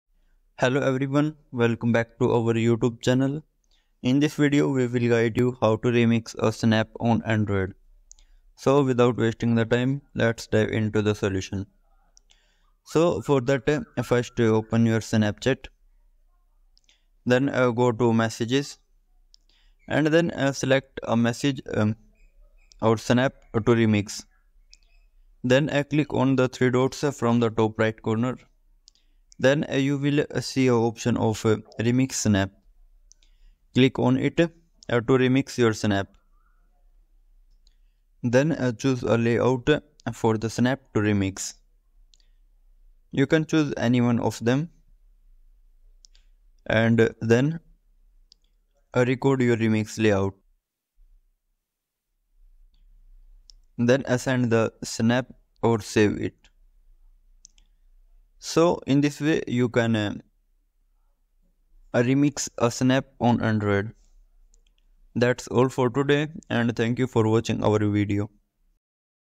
How To Remix A Snap On Android
In this tutorial video, you will learn how to remix a snap on Android. The video will guide you through the process step by step, making it easy for you to create your own unique remixes using your Android device.

00:12  Steps To Remix A Snap On Android